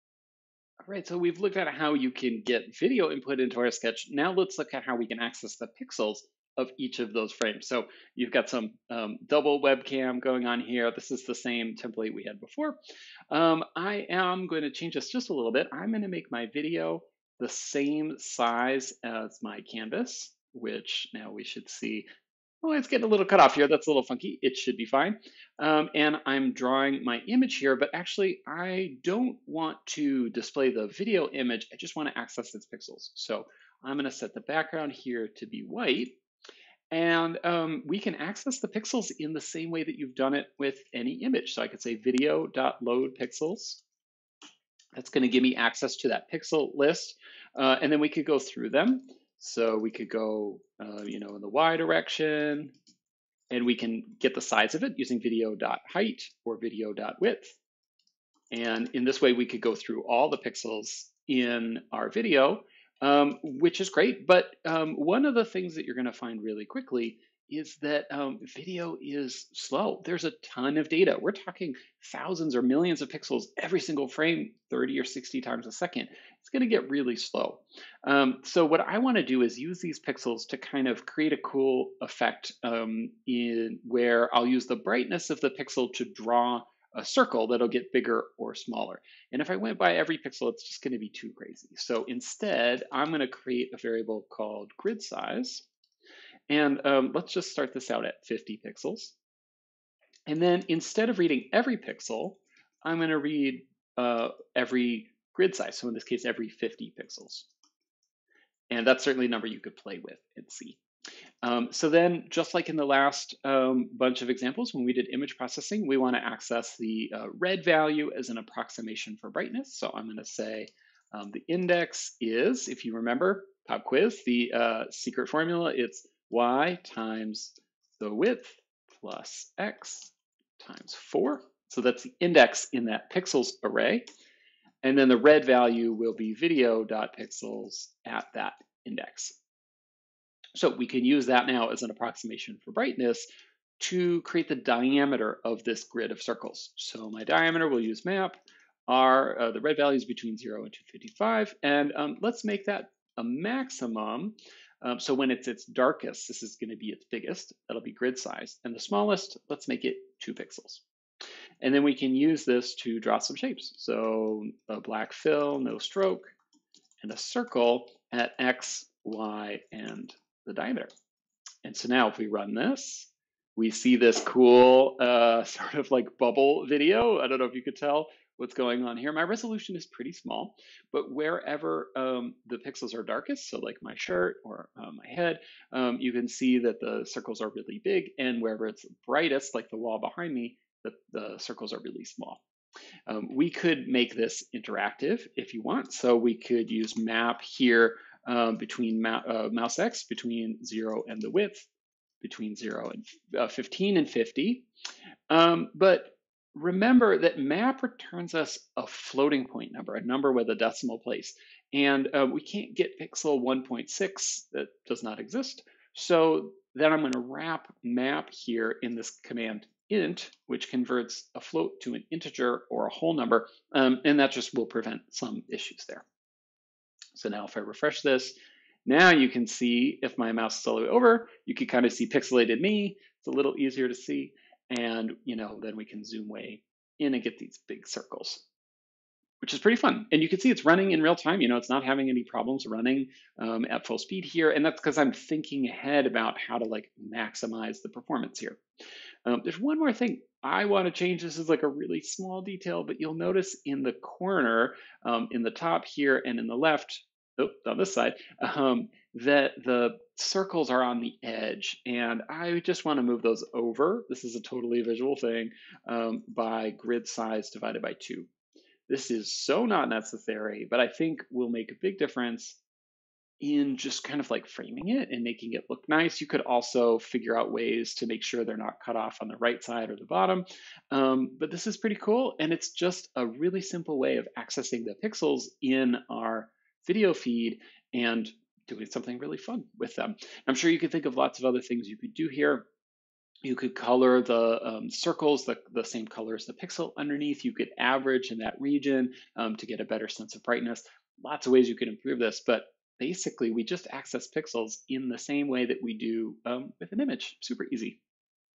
CP2: Reading Video Pixels – Webcam Tracking in p5.js
Video input is basically just like an image! We can get it's width and height, and access the frame's pixel values too!

CHALLENGES
❓ Can you have the circles filled with the pixel color instead of just black?
❓ Notice a lag before the video starts? Can you have something else display onscreen before the camera is ready? (Hint: if video.width === 0, then it's not ready yet)